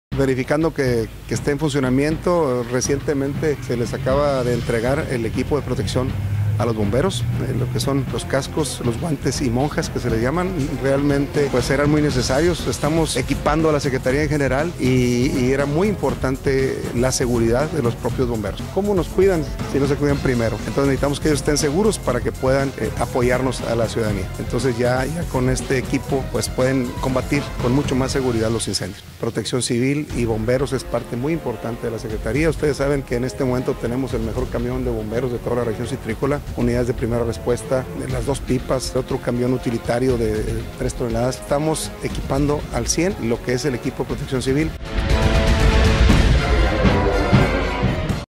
ENTREGAN EQUIPO A ELEMENTOS DE PROTECCIÓN CIVIL
Allende.- El alcalde Ing. Silverio Flores Leal acudió el día de hoy a las instalaciones de Protección Civil para entregar cascos, guantes y monjas a los bomberos del municipio.

Esto con el fin de que puedan combatir de una mejor manera las emergencias que se lleguen a presentar en el municipio, así como también para asegurar la seguridad de los bomberos.